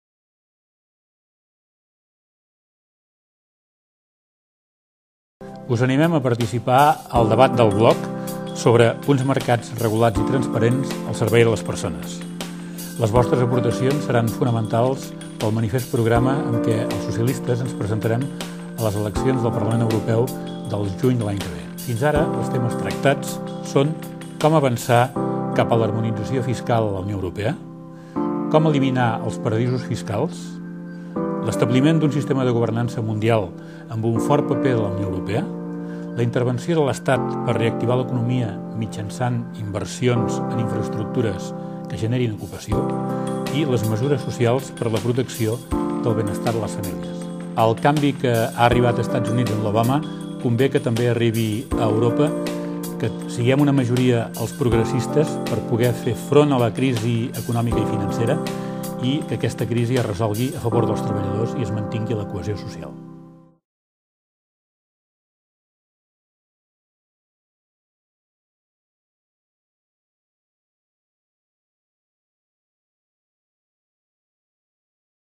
1er Debat "Activa't per Europa": Uns mercats regulats i transparents pensant en les persones
El diputat del grup Socialistes-CpC al Parlament de Catalunya, Josep Casajuana i Pladellorens, obre el primer debat del bloc "Activa't per Europa", que porta com a títol 'Uns mercats regulats i transparents pensant en les persones'.